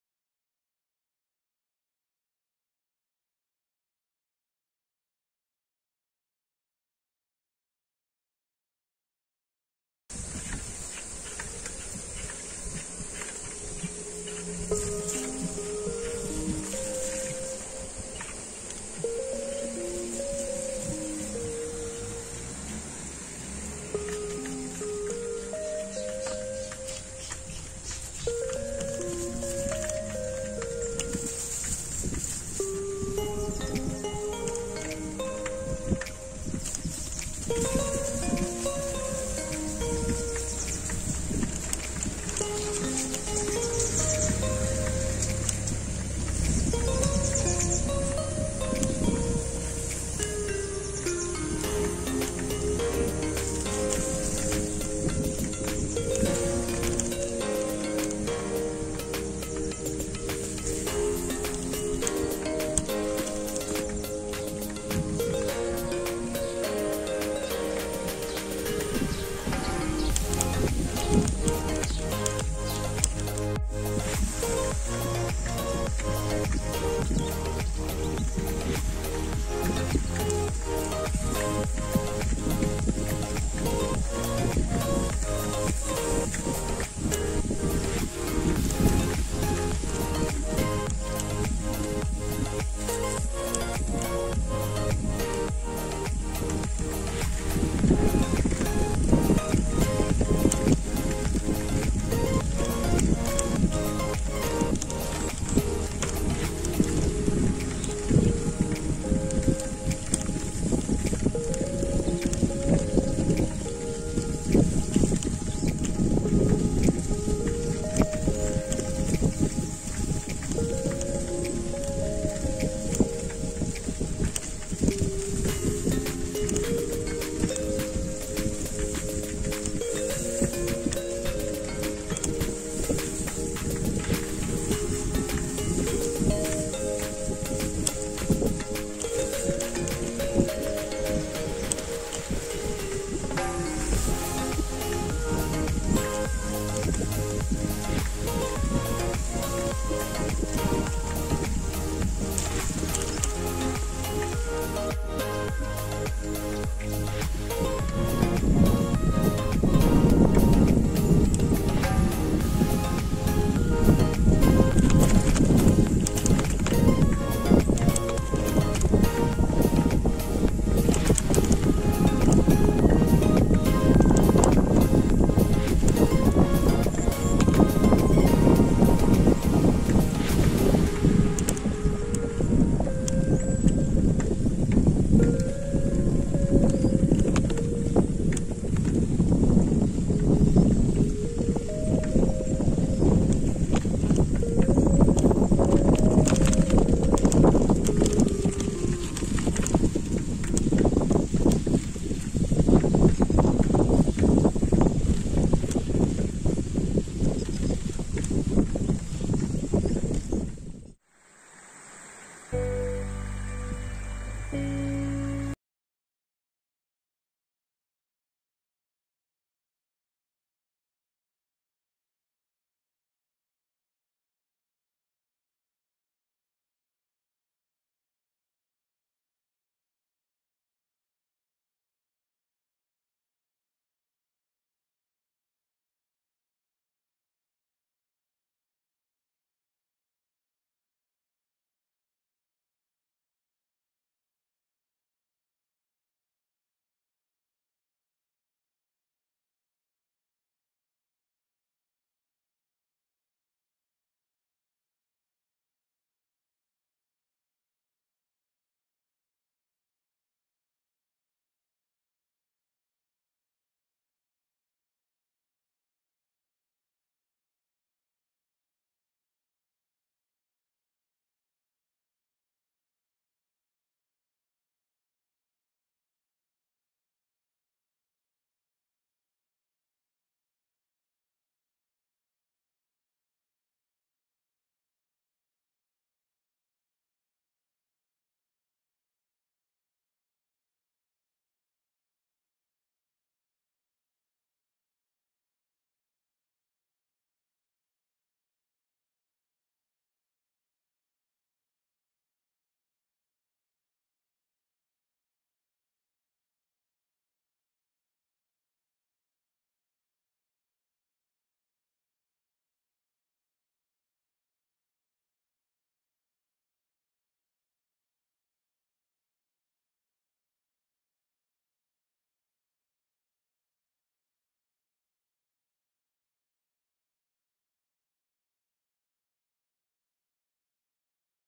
A solo ride to Rayang | Besides Rayang Military Station | Beautiful landscape | Jonai cycling vlog
Hello guys in this video I'll be showing you a place called Rayang on the outskirts of Jonai (my hometown). As a cycling enthusiast, I love pedalling around and seeing new places and recording videos of them.